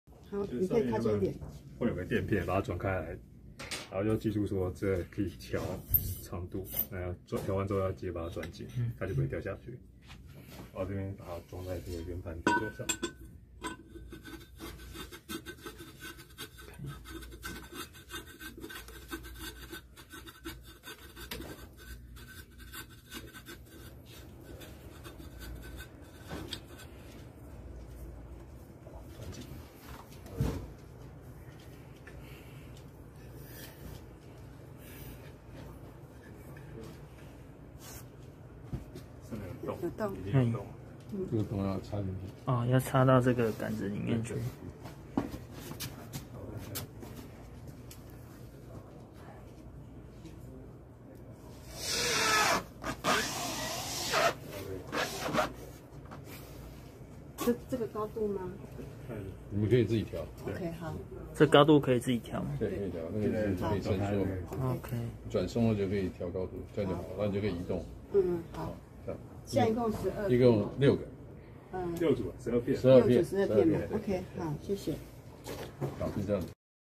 2023年1月26日
How to install the Auralex ProMax V2
to a stand。